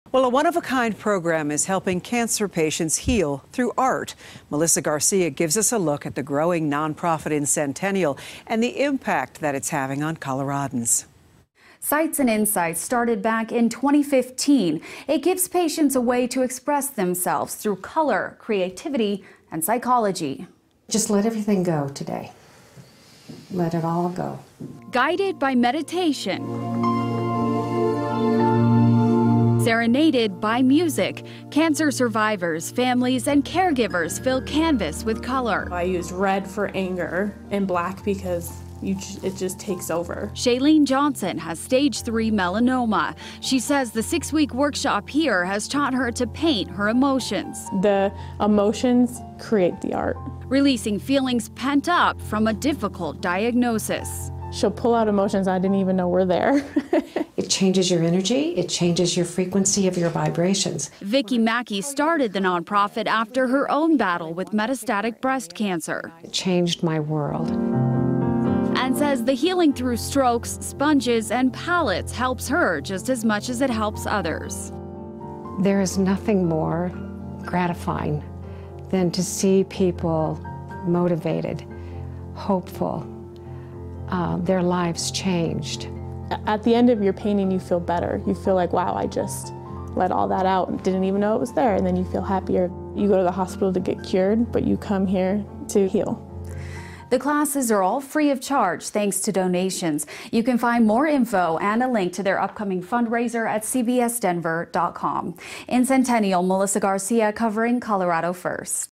Sites And Insights Offers Art Therapy For Cancer Survivors
A Colorado non-profit organization is helping to heal cancer patients through art. Sites and Insights in Centennial puts on six-week workshops with a unique curriculum involving color, creativity and psychology.